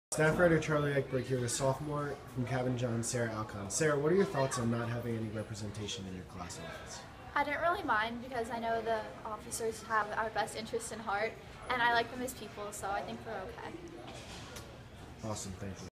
Freshman from Cabin John Unrepresented in High School SGA
filmed by Charlie Eichberg